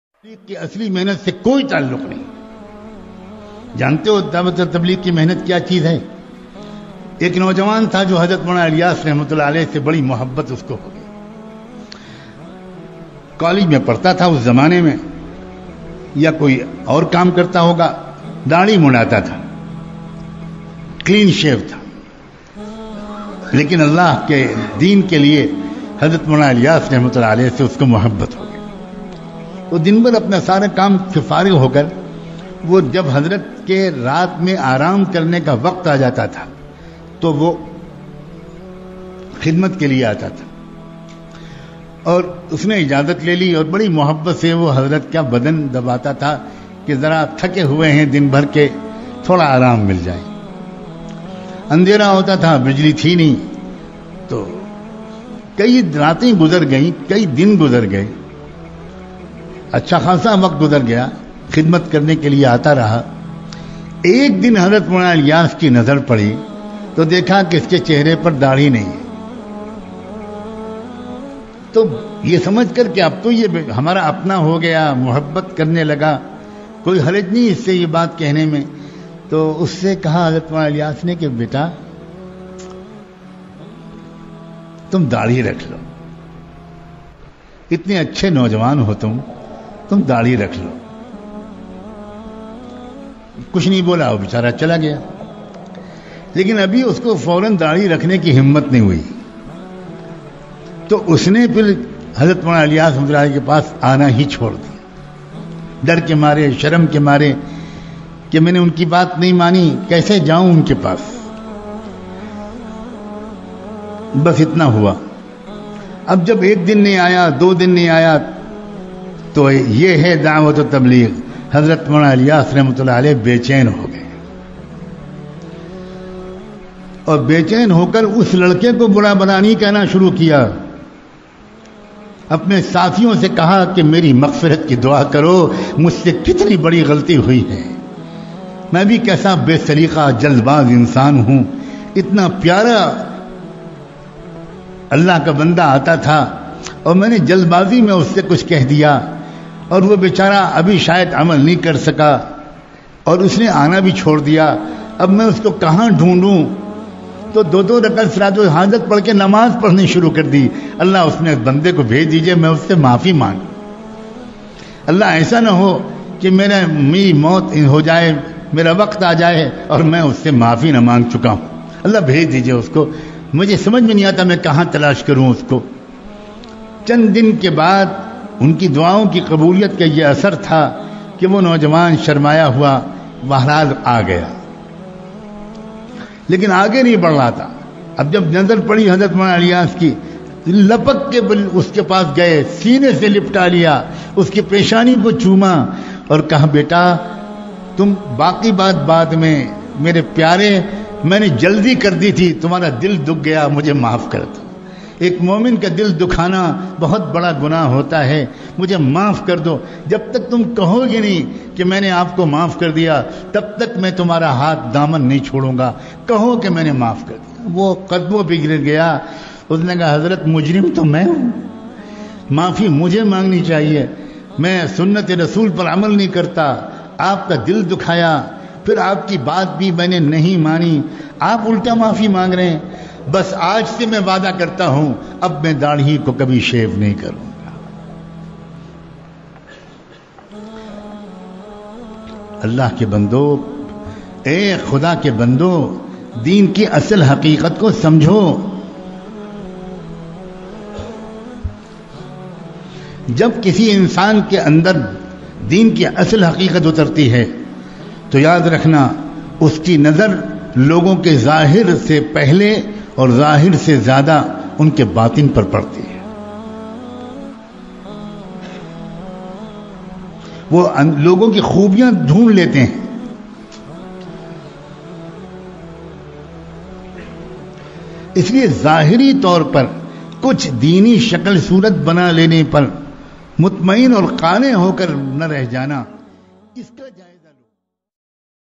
Moulana Ilyas Sahab RA aur eik college student ka waqia__Moulana Khalilur Rahman Sajjad Nomani
Moulana Ilyas sahab RA ka college student ke saath eik waqia.
Tablighi jamat karguzari